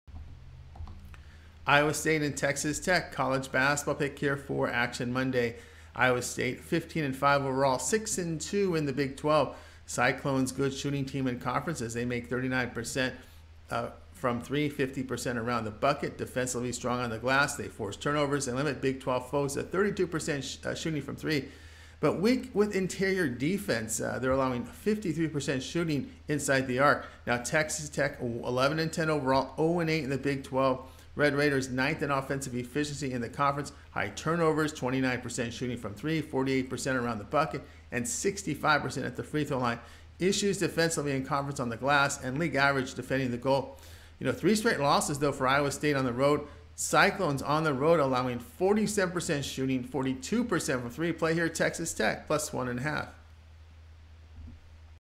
Iowa St vs. Texas Tech 1/30/2023 FREE College Basketball Picks and Tips on NCAAB Betting Tips
1/30/2023 Iowa St vs. Texas Tech FREE College Basketball Picks and Tips on NCAAB Betting Tips for Today